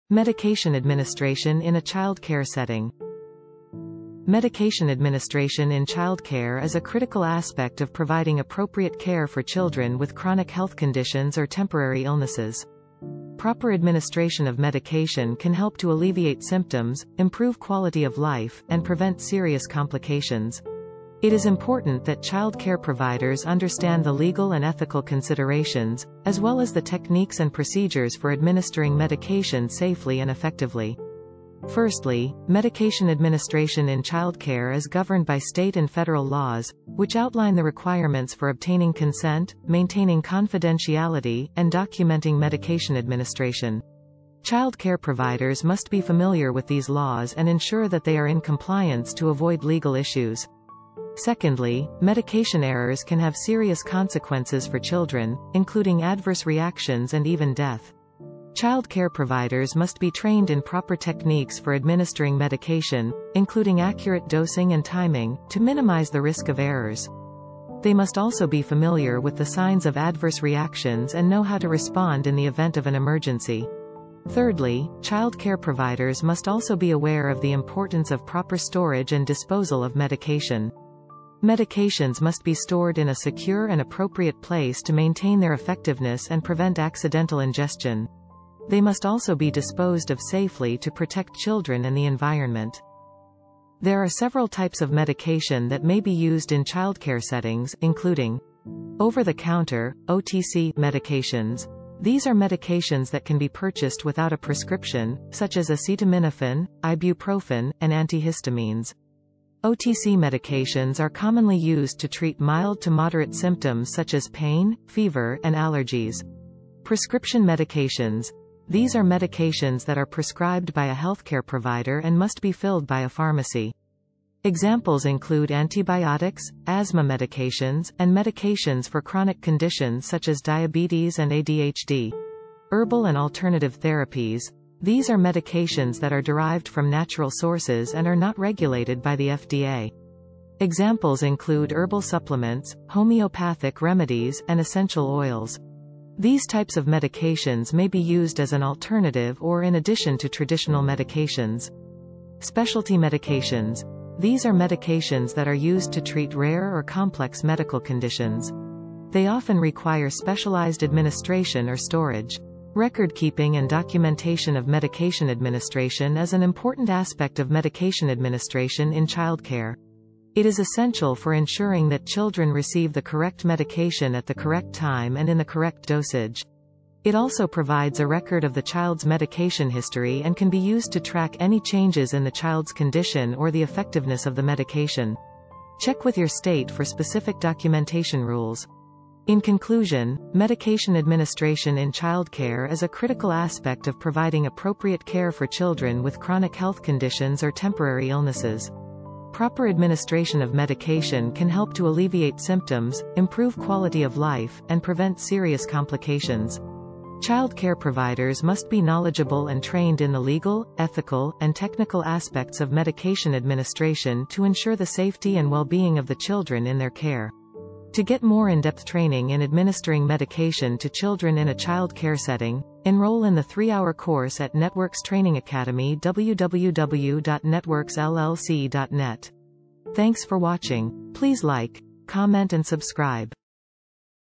Medication Administration in a Child Care Setting
Medication administration in child care is a critical aspect of providing appropriate care for children with chronic health conditions or temporary illnesses.

for the 3 hour continuing education course in medication administration,  contact